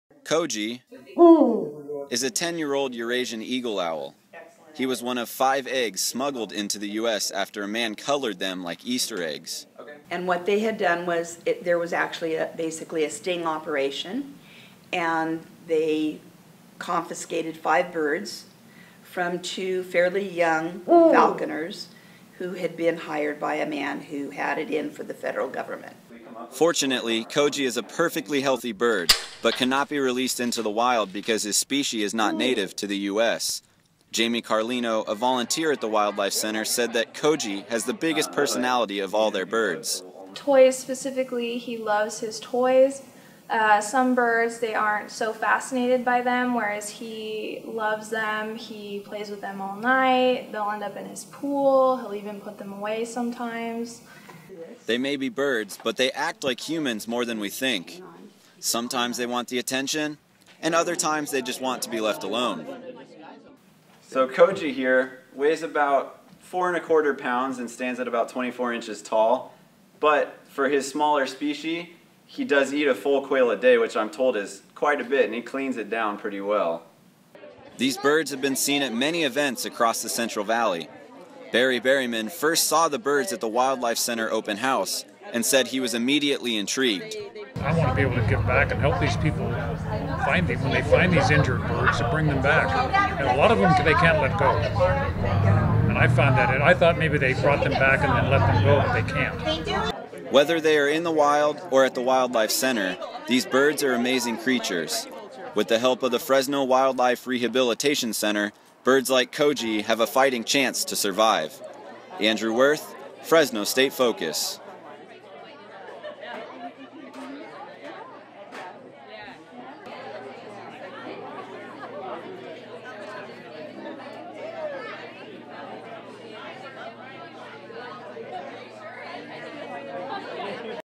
Fresno Wildlife Center
A project produced in the Fall 2015 MCJ 128 News and Public Affairs Production Class for Fresno State Focus.

Produced: November 16, 2015 by Andrew Worth.
Instructor: Faith Sidlow

This story looks into the life of one of the birds being cared for by members of the Fresno Wildlife Rehabilitation Center.